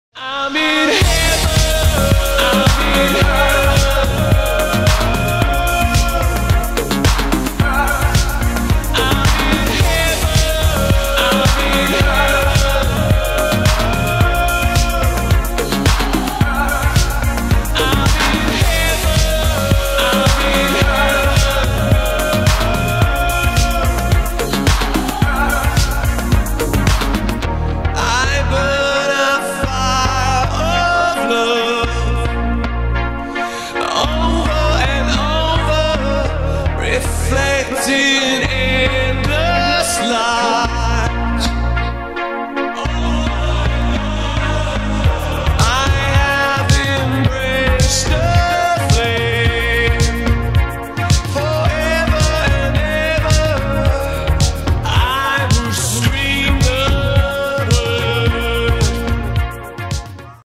Broadcasting from vinyl record
We were broadcasting from vinyl on a small, local, non-profit station :) (in the past we did broadcast from tape as well, once from my Revox B77 and later from my Studer A807)
The sound was taken from the internet stream rip. (which is fed from the digital output of the TC DBMax broadcast processor)
The Behringer compressors were not in the music signal path, they were used for the microphones only.